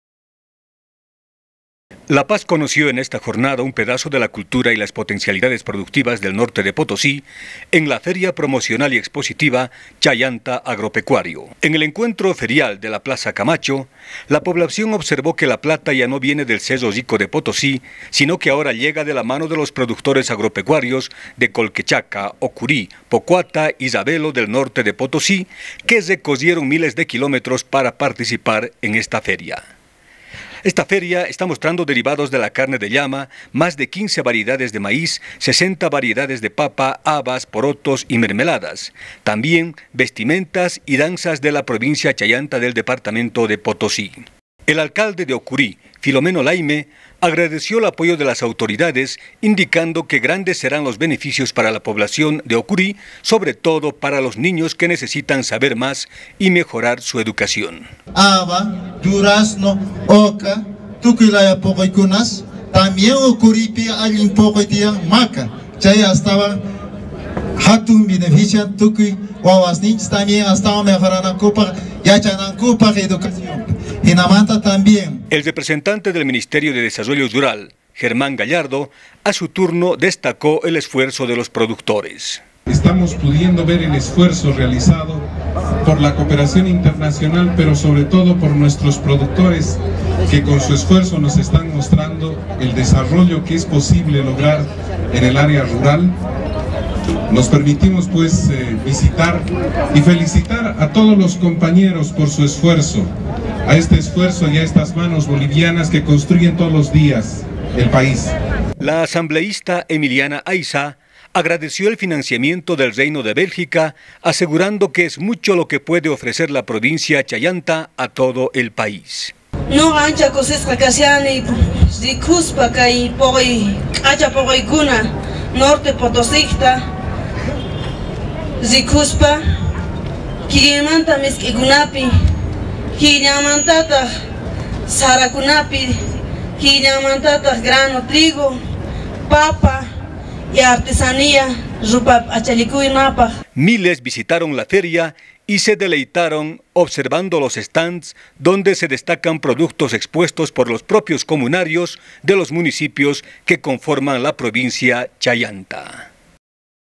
Feria Exposición Chayanta Agropecuario
El viernes 20 de Mayo de 2011 en la Plaza Camacho de la ciudad de La Paz se llevó a cabo la Feria Promocional y Expositiva "Chayanta Agropecuario". La feria mostró derivados de la carne de llama, más de 15 variedades de maíz, 60 variedades de papa, habas, porotos y mermeladas, vestimentas y danzas de la Provincia Chayanta del departamento de Potosí.